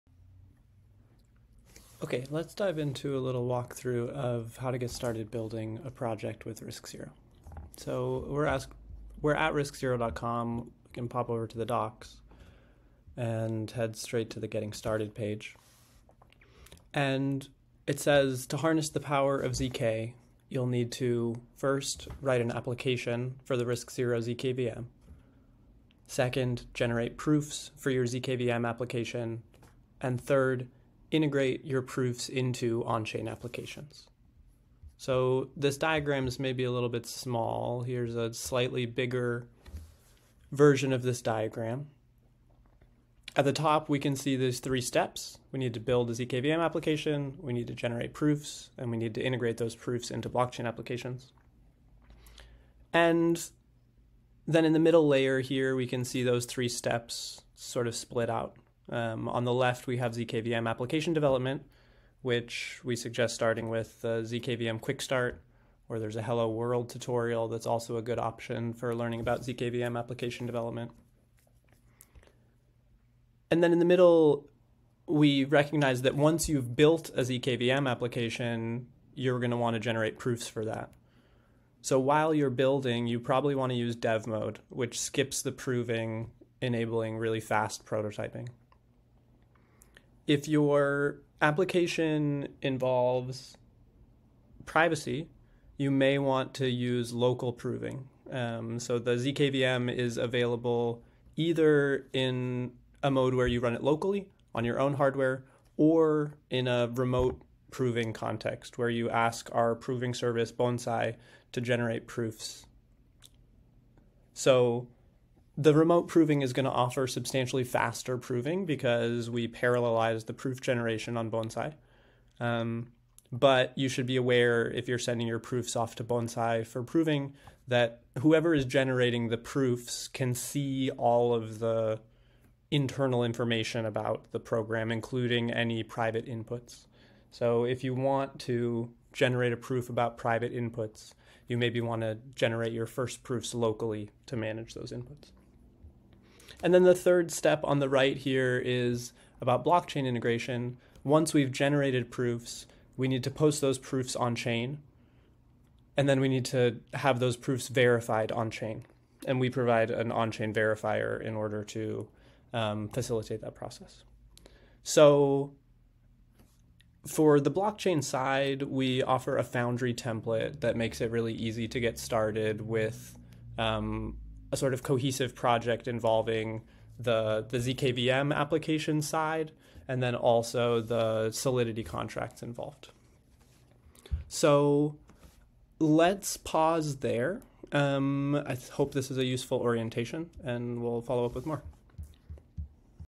Getting Started with RISC Zero
A quick overview to building a project with RISC Zero.